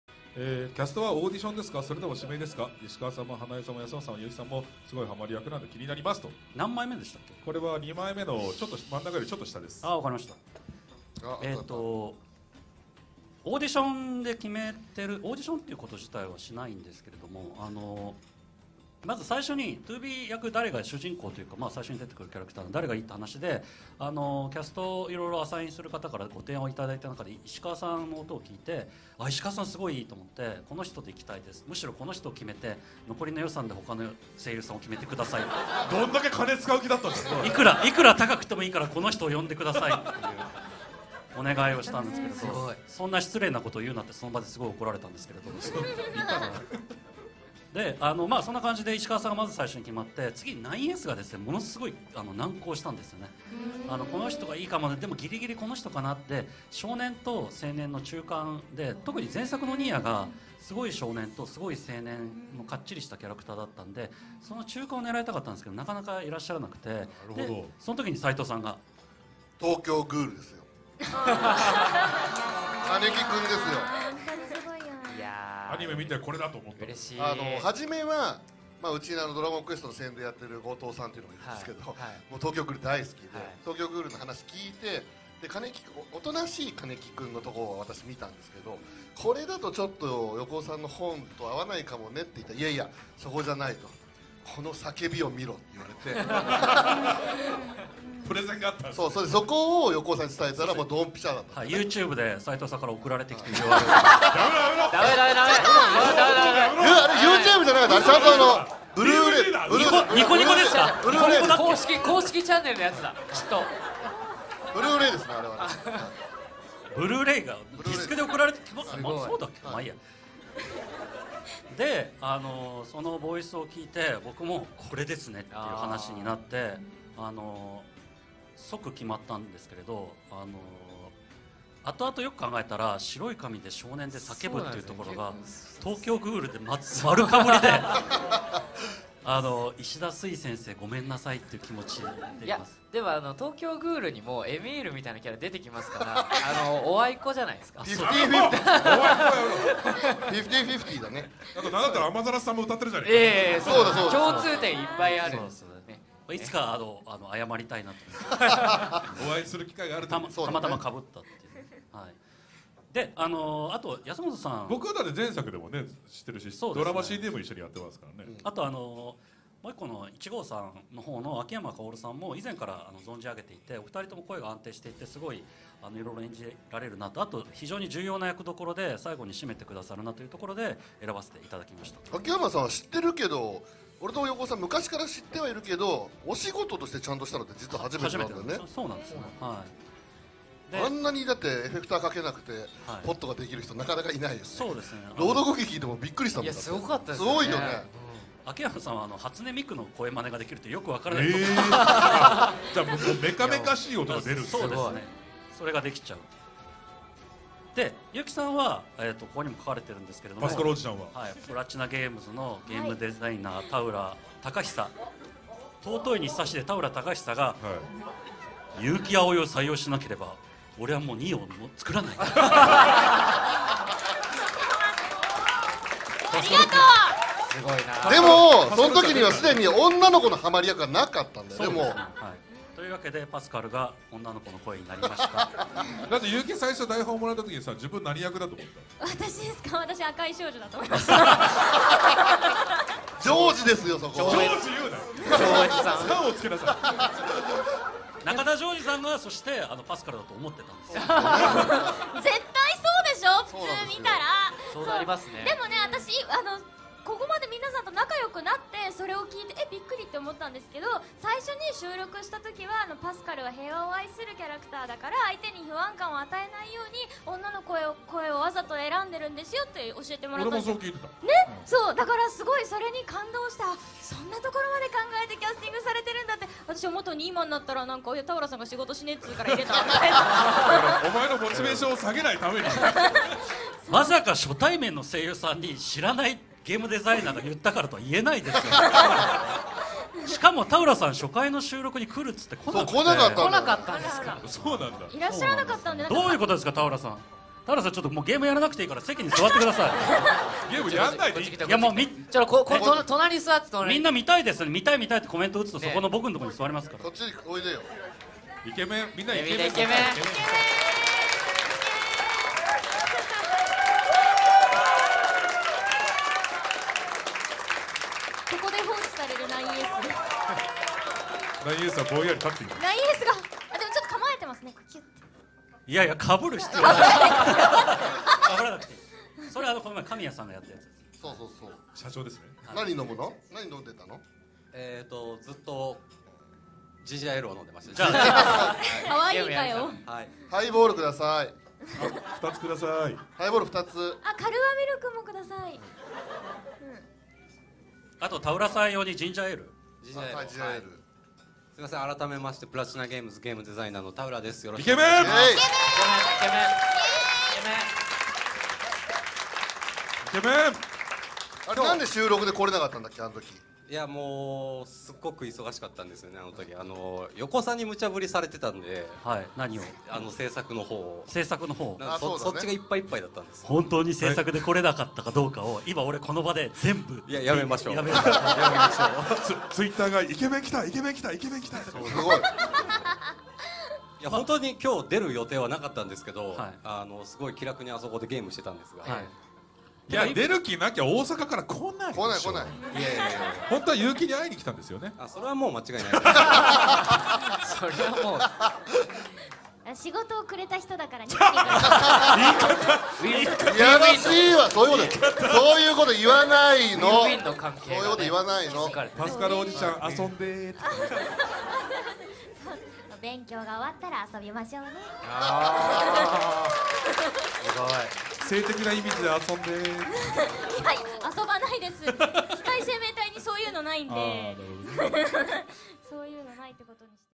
How Ishikawa Yui, Hanae Natsuki, Yuuki Aoi, Yasumoto Hiroki got their roles in NieR:Automata
Game Director Yoko Taro explains on how he casts the roles in NieR Automata casts.

Video is from stream of the event Spoiler Dinner Talk Show@STORIA.